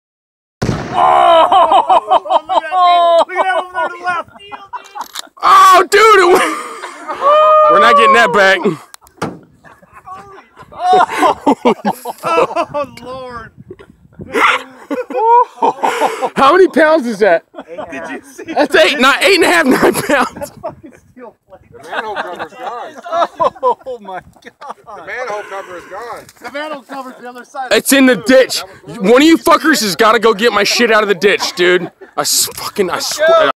8 1/2 pounds of tannerite vs.
40lbs manhole cover is flying to the left 70-80lbs 1" steel plate flew 50 feet and cleared the ditch to the the right.